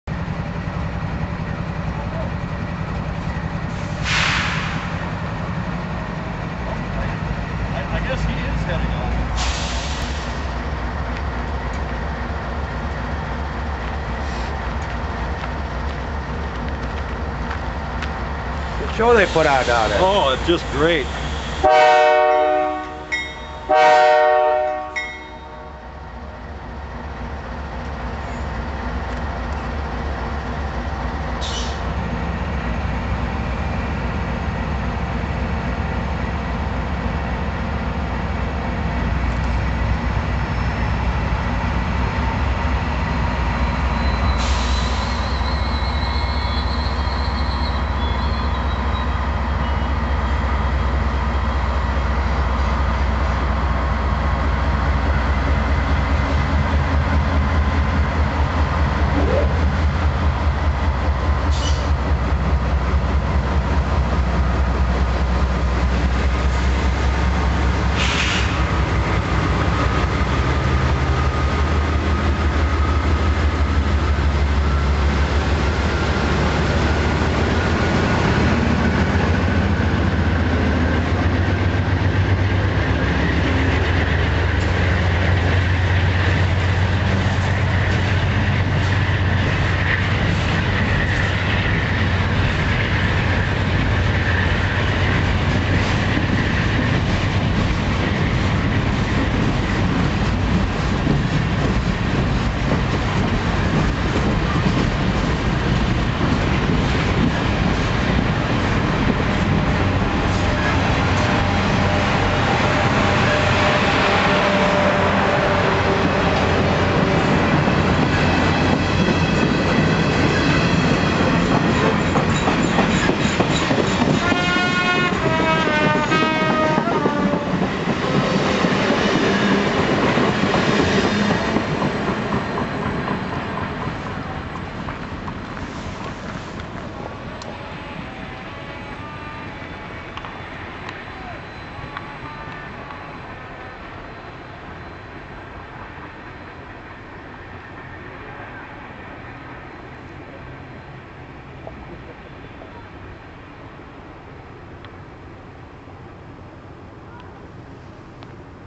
Canadian Pacific Holiday Train, Columbus, WI 8 Dec 12.avi
CP 9824 leaves the Amtrak station at Columbus WI. with the Canadian Pacific Holiday Train in tow.  They will tie down at Portage.  If you ever get the chance, go and see this train.  Per the engineer, they have collected a lot of food for the needy.